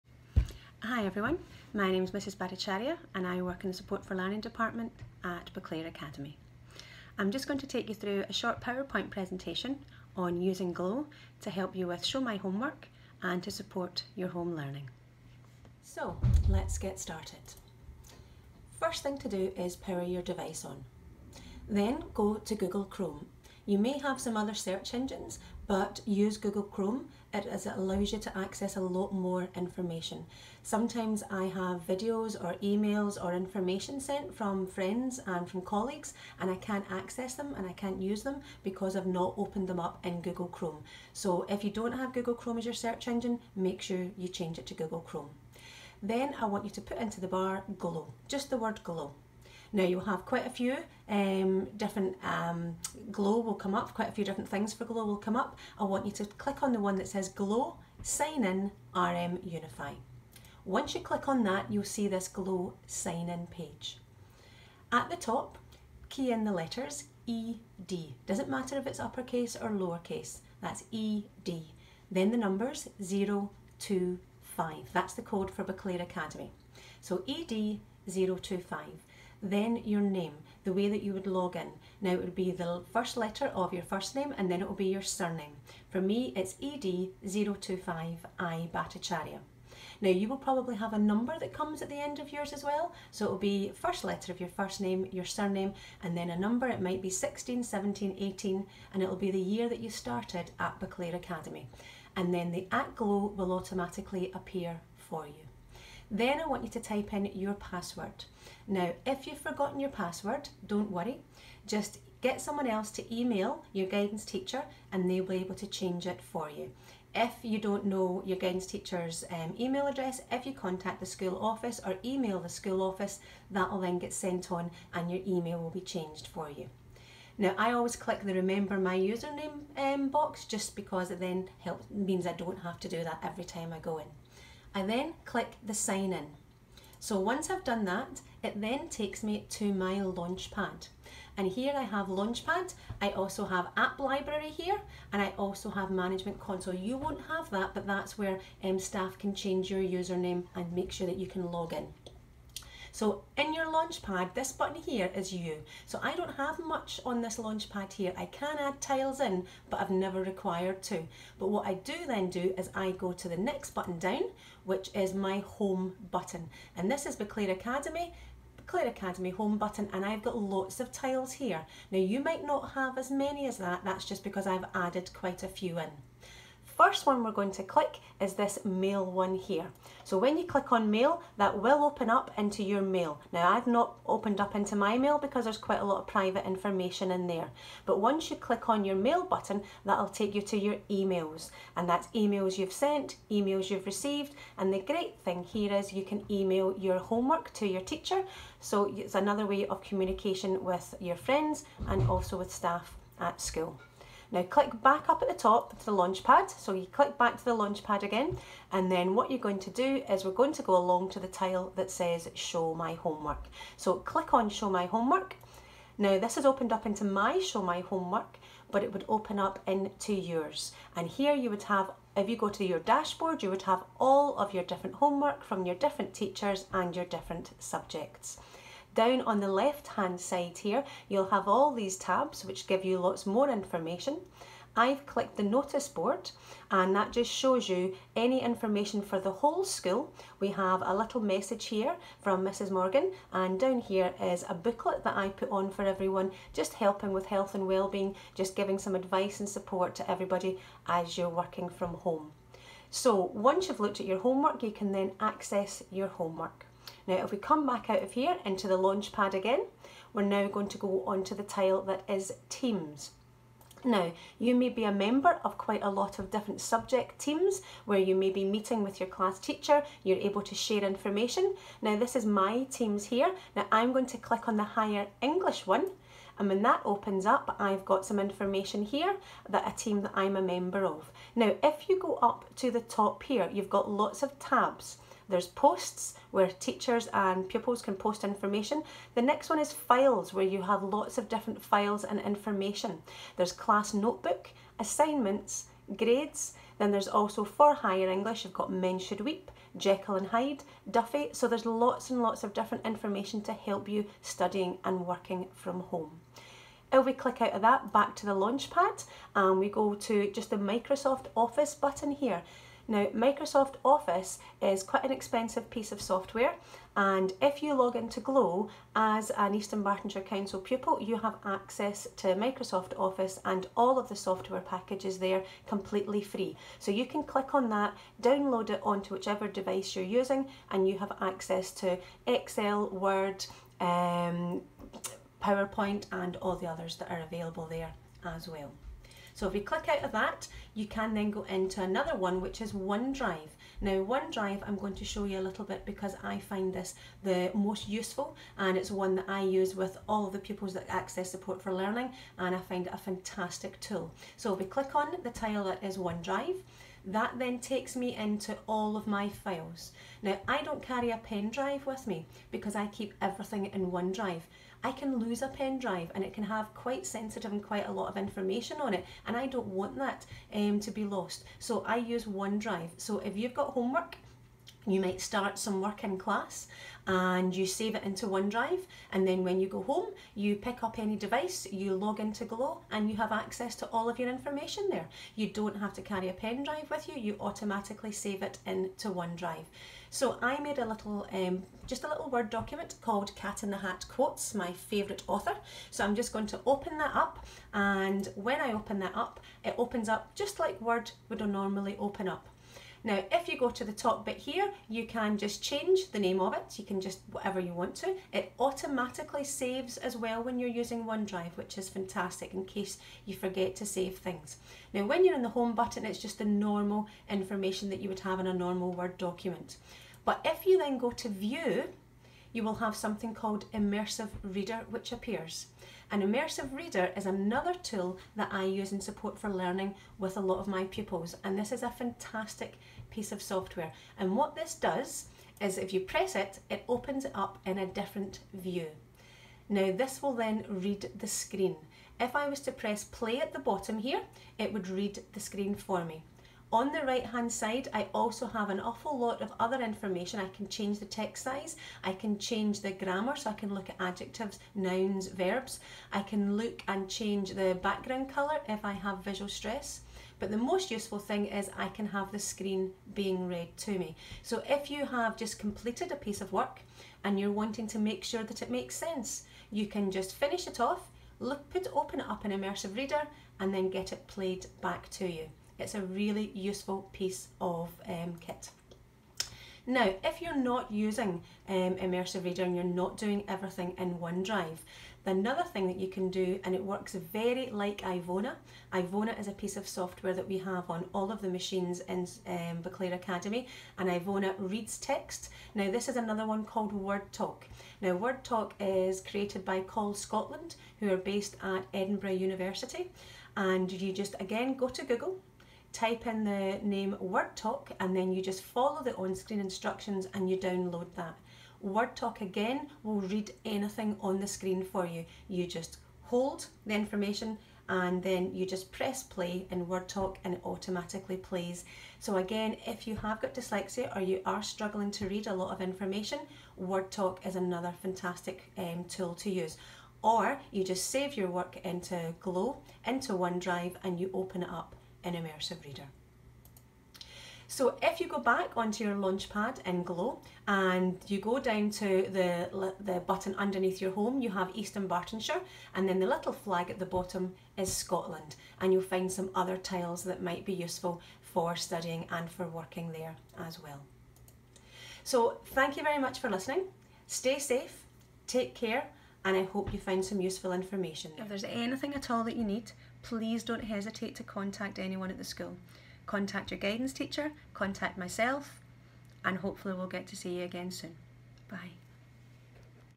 SFL & WB Boclair Academy
Step by Step Guide to logging into GLOW, SMHW, TEAMS and One Drive